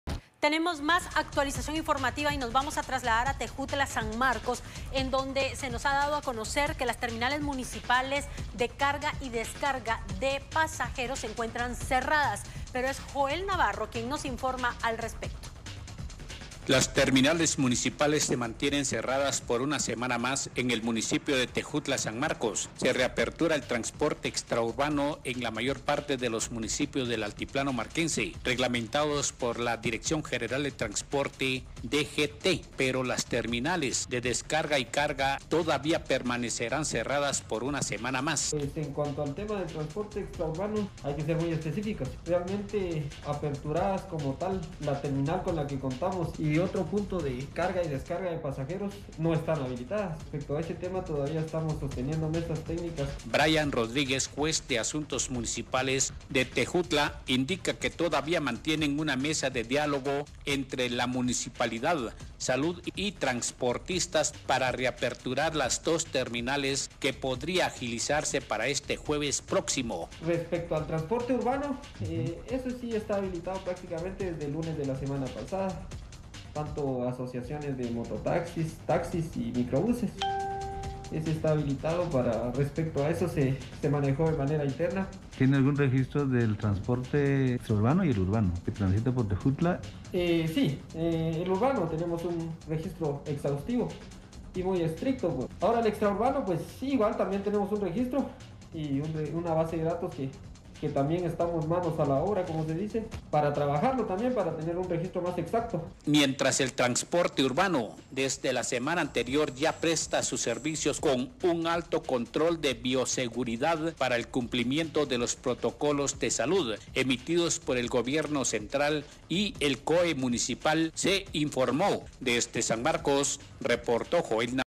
Terminales de carga y descarga en Tejútla permanecen cerradas
Se sostienen mesas técnicas con trasportistas

Las Terminales municipales de carga y descarga se encuentran cerradas por una semana más en el municipio de Tejutla, San Marcos. Visita nuestro sitio